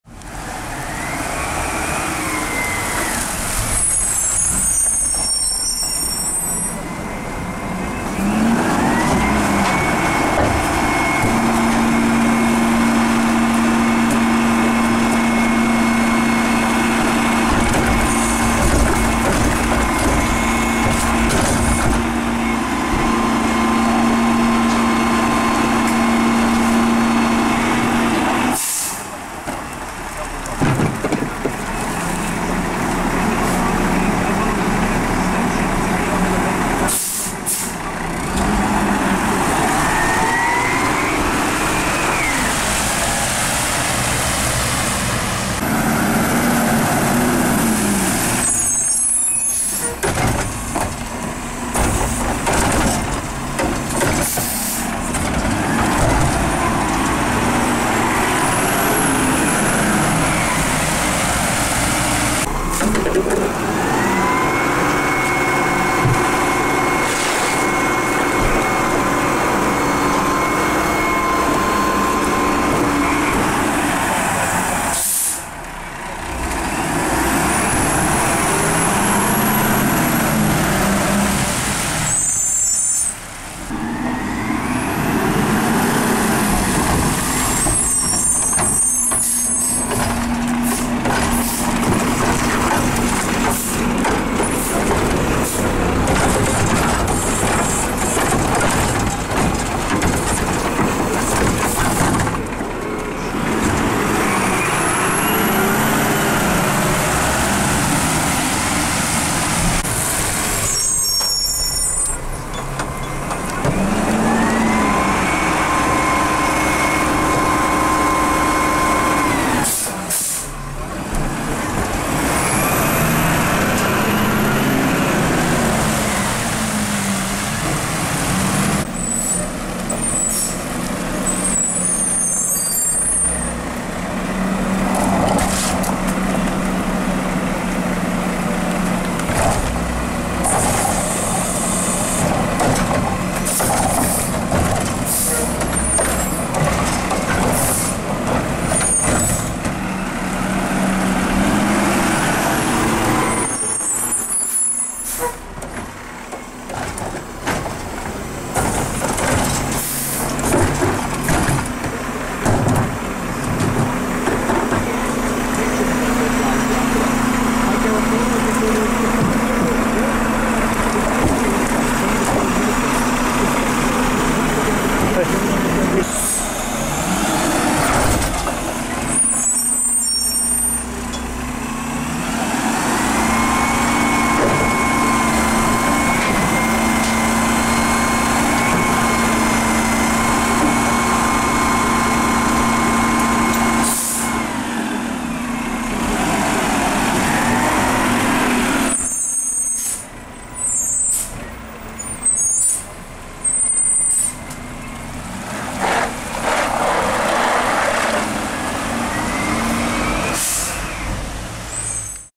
Mack LR Heil Curotto Can Dumping Manual Bins
E.L. Harvey (now a Waste Connections company) uses these trucks to pick up customer-provided cans in the town of Lexington, Massachusetts. Thanks to the driver for putting on a good show!